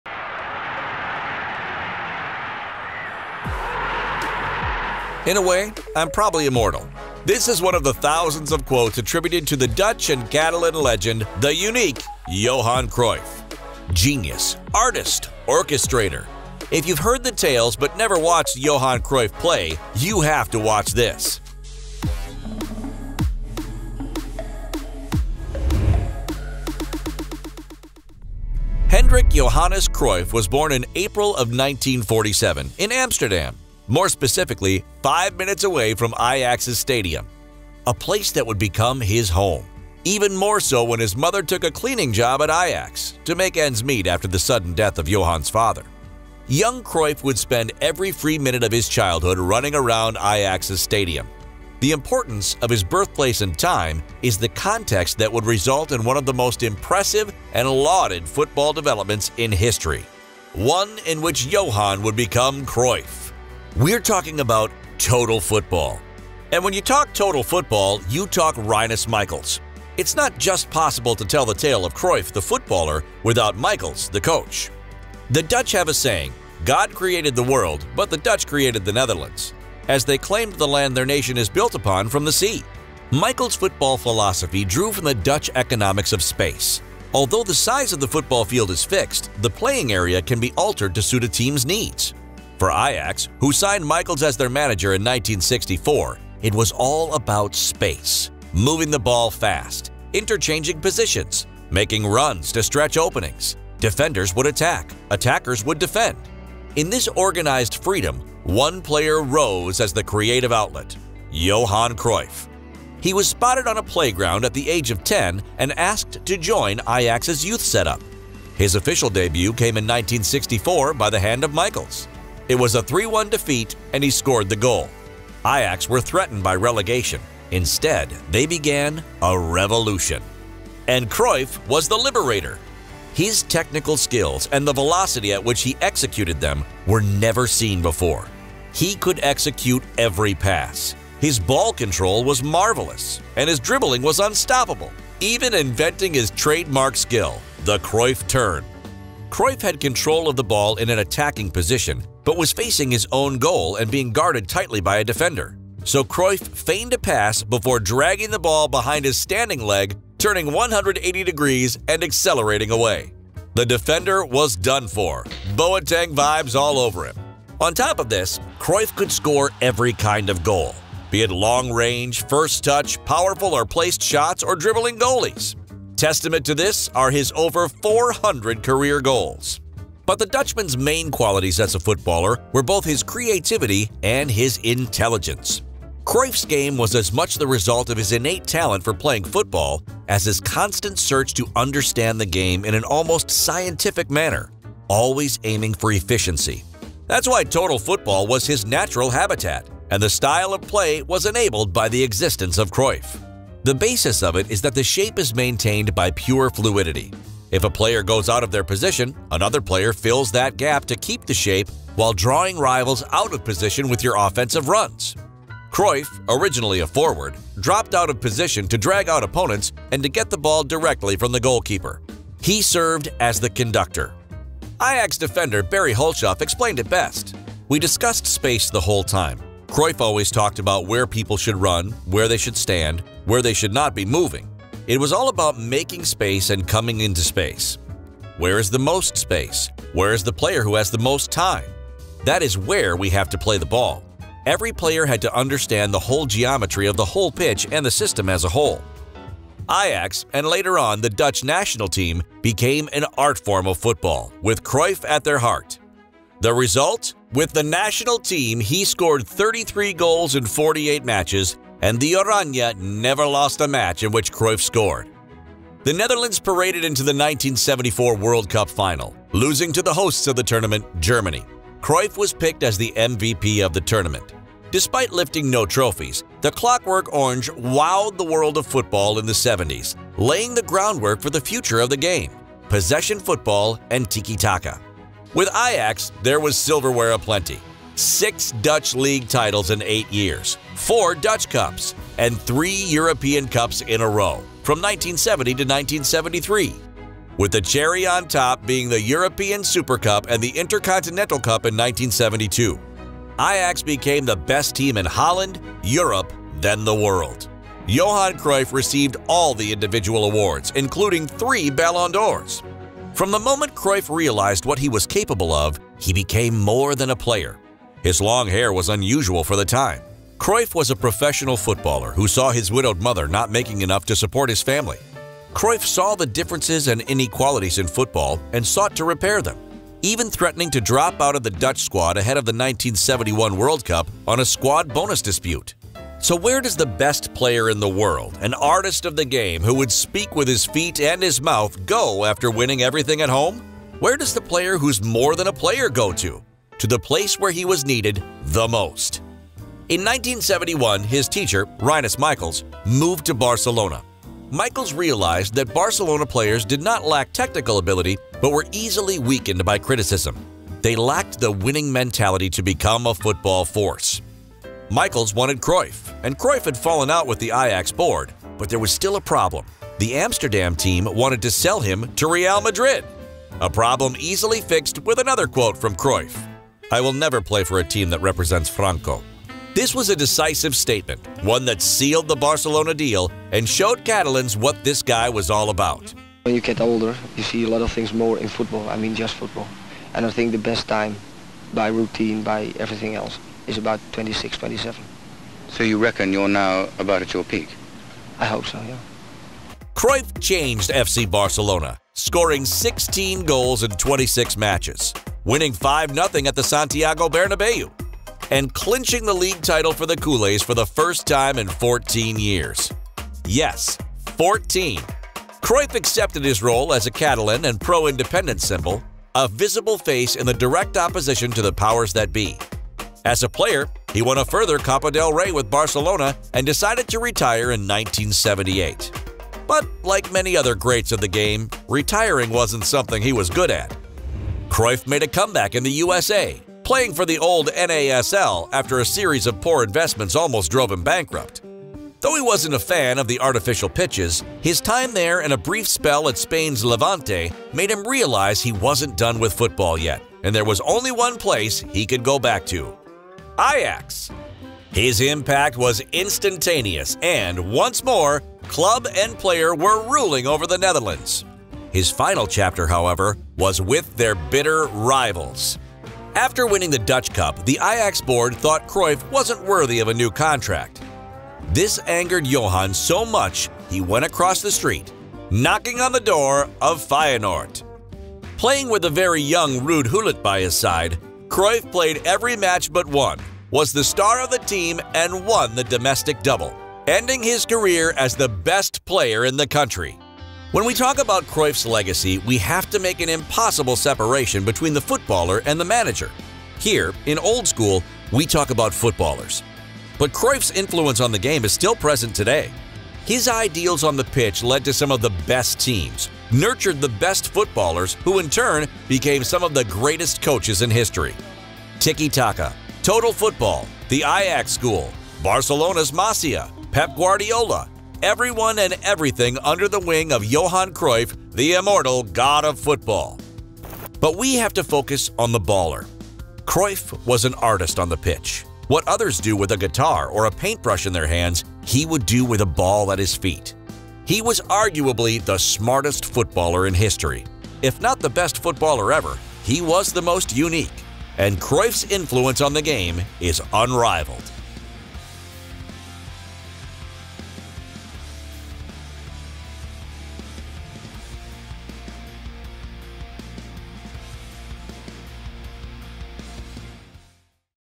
Was Johan Cruyff Really As Good As People Said He Was?
It’s time for Old School to take a look at one of the most creative players in history, Johan Cruyff, the Dutch legend who changed football forever! From his start at Ajax, the total football of the clockwork orange and the revolution at FC Barcelona, Johan Cruyff was one of the most unique characters in the game we love. If you’ve never seen Johan Cruyff playing, you have to watch this video!